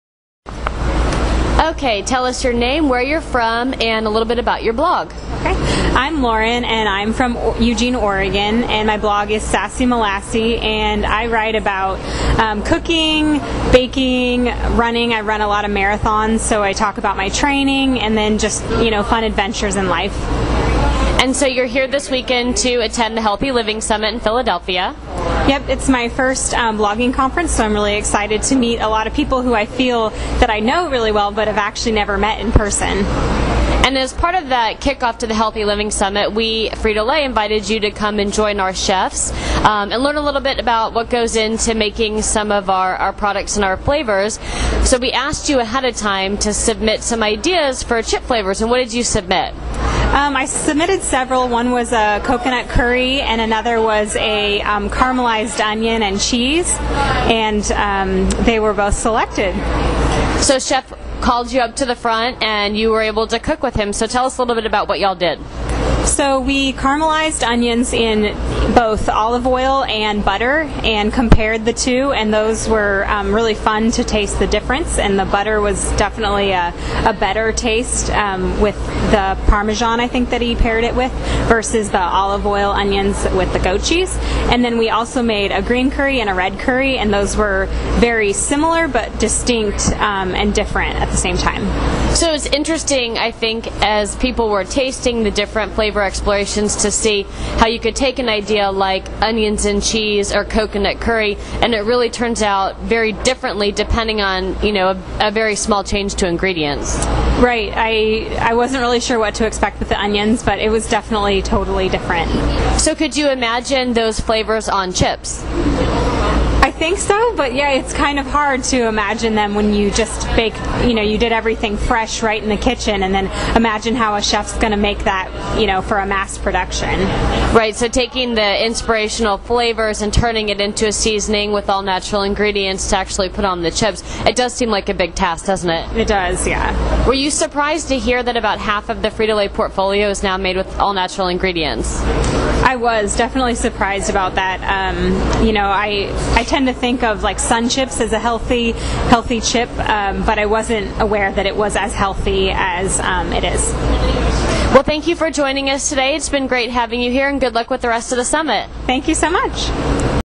Snack Chat™: Healthy Living Summit Podcast with Sassy Molassy
Kristin Holmes, Frito-Lay Sr. Manager of Communications, interviewed blogger Lauren from 'Sassy Molassy' about her experience at the 2011 Healthy Living Summit, in which Frito-Lay brought to life its Flavor Kitchen™ through a cooking demonstration using real foods and flavors to create recipes that could inspire new flavors of snack chips.